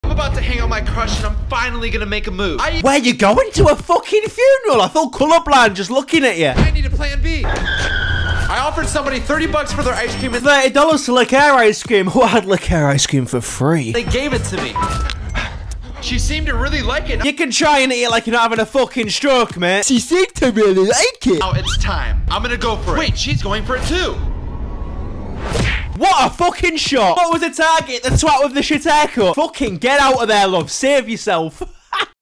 Lexi Riveras Boyfriend Is So DUMB 🤣
Jeremy Hutchins uploads a video of him trying to kiss his crush, who is Lexi Rivera. I had no idea that Brent Ribera had a sister, so this is my reaction to this very cringe video.